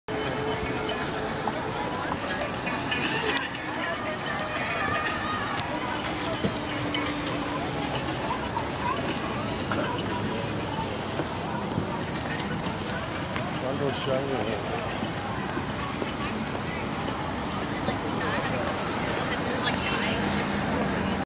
Downtown Vancouver
Walking around the heart of Vancouver